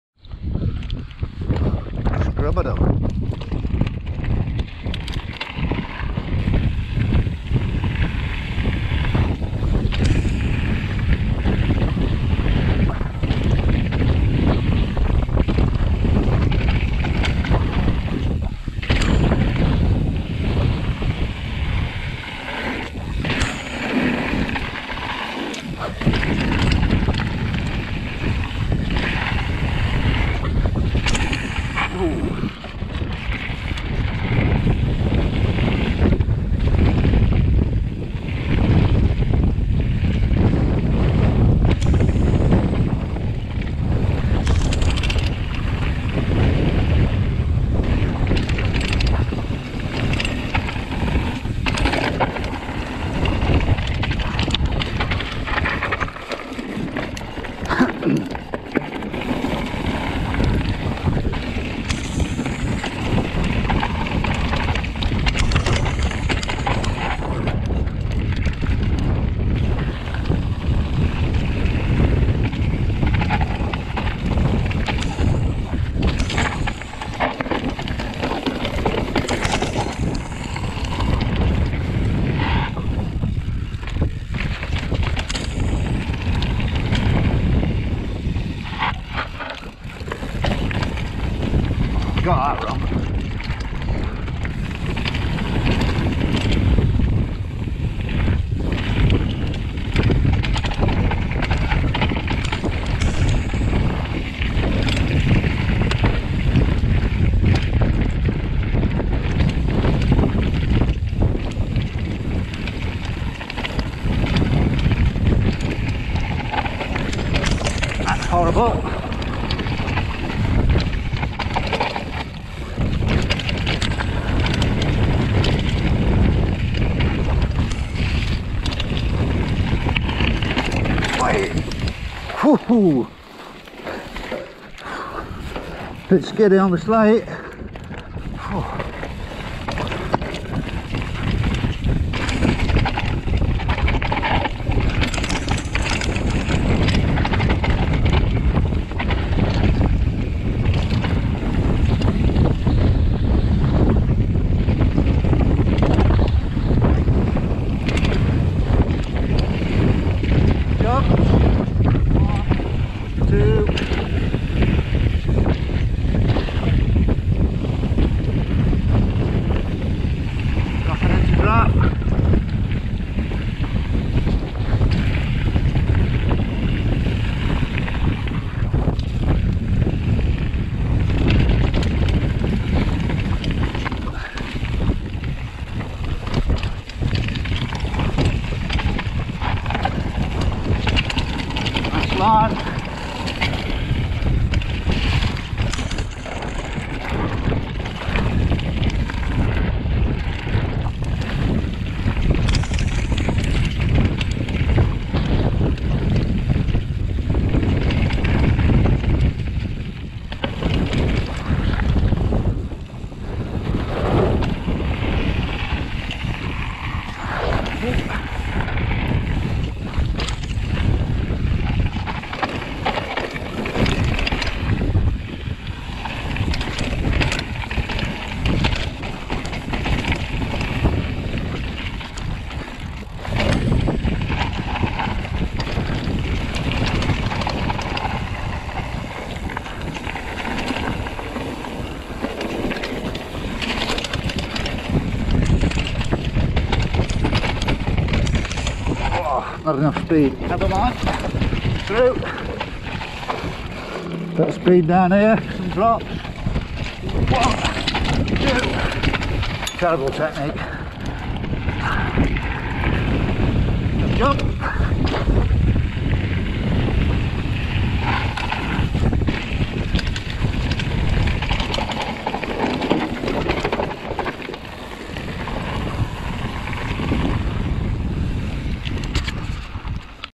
Antur Stiniog - Scrubadub- First run. Sonder Signal Ti MTB
Sketchy run down Scrubadub at Antur Stiniog Bike Park on my Sonder Bikes Signal Ti hardtail. Great place to ride, bit hair raising in the damp though 😂